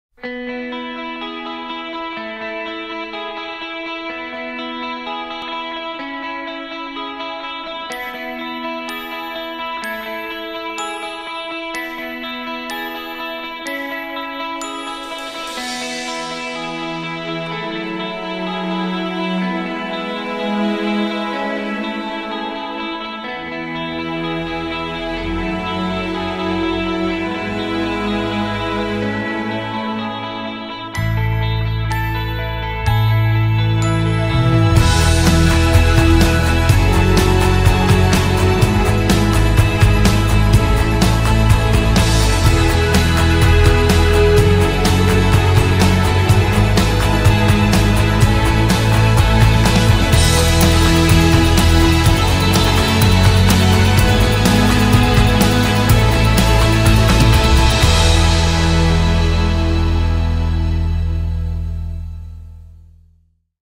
Twin mishap| GCMM| Original concept Trailer
I hope you will enjoy the GCMM it took days! Weeks!! To make!! And it had like 1,678 photos. And when editing it it was so glitchy but it looks amazing so I hope you like it!! 😁